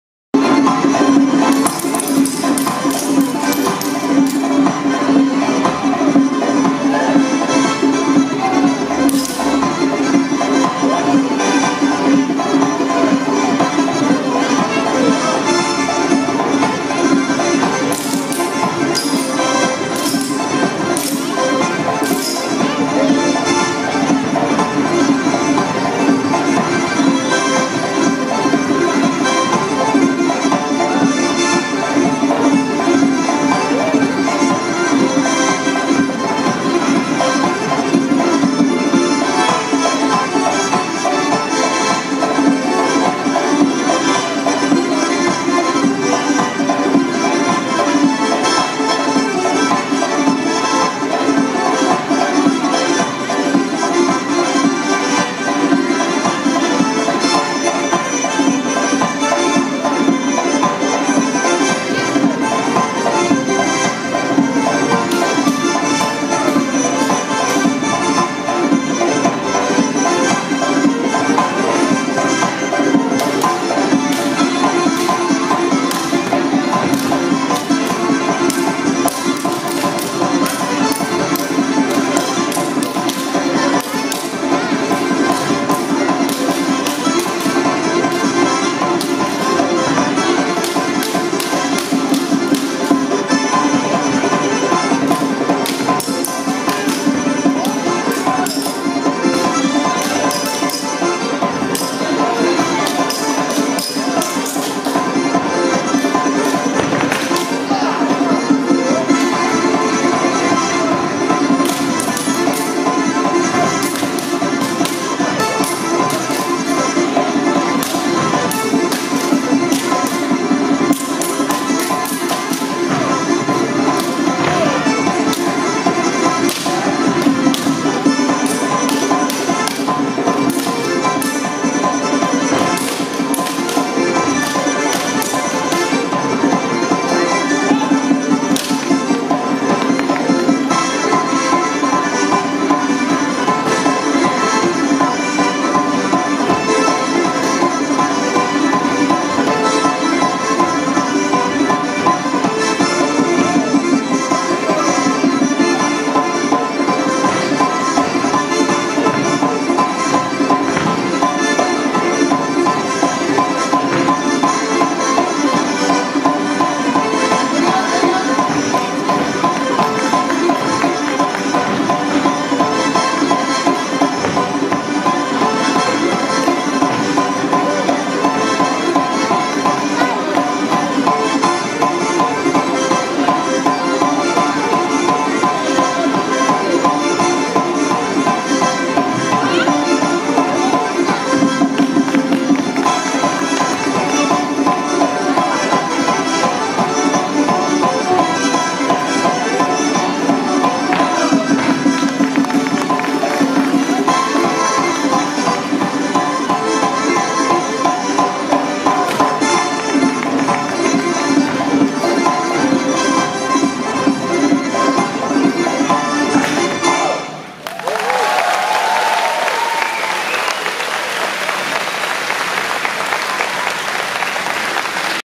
Tsiskari, Georgia #3 at 50th OpRoakeldais 2015
Tsiskari is a dance group from Georgia performing here at Warffum, Netherlands
P1030446